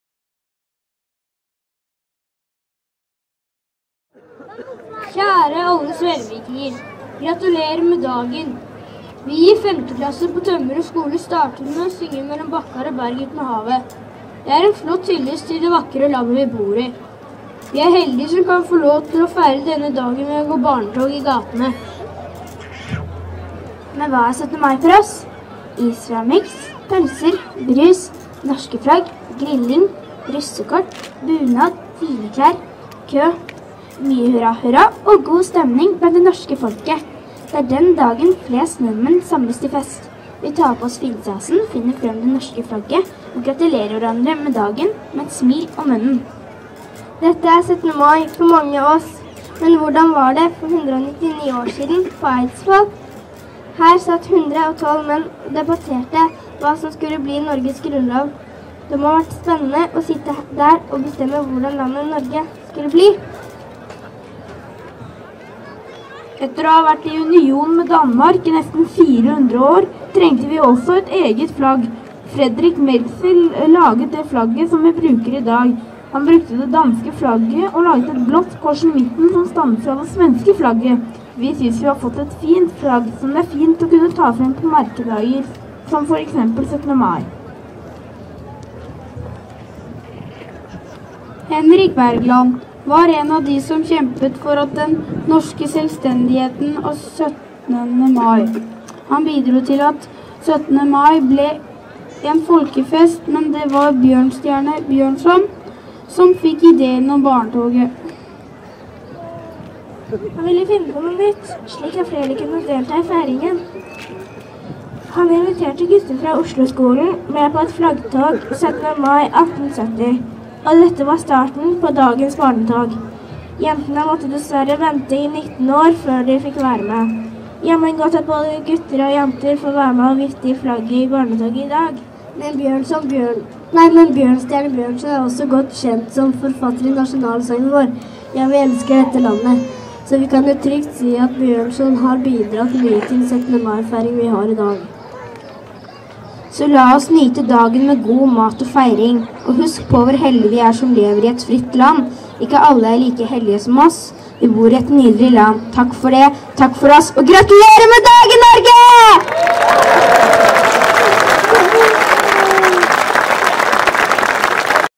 17.mai tale
Femte klasse ved Tømmerås skole holdt årets 17.maitale. En flott tale ved Dampskipsbryggen ved Svelvikstrømmen i Svelviks nye storstue Amfiet i Kirkeparken.